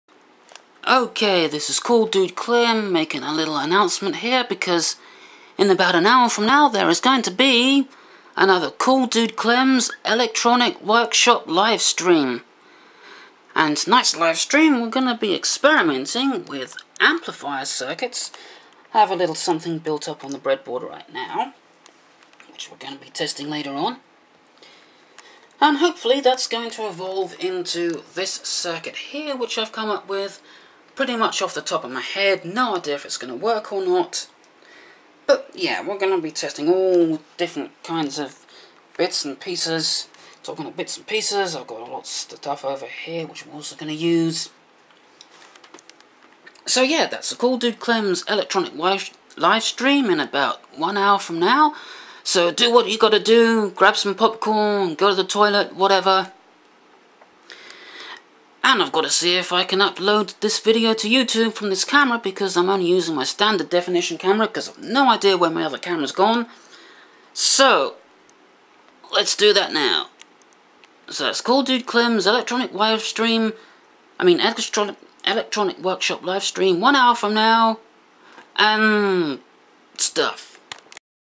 CCD Electronic Workshop Livestream in one hour anouncement thing.
How about trying to build an amplifier from scratch? (well I already have the output stage built because if I didn't, the live-stream would take forever) Anyway I'm sure lots of things will go wrong but I'm sure I can fix any problems that might come up and get the circuit working. Who knows ... it could be a good amp.

And yes it appears that YouTube accepts MPEG MOD files that this camera makes, really I dont know why the files it creates have the MOD extension, to me MOD is an old Amiga music tracker format.

I hate the way my voice keeps squeaking here.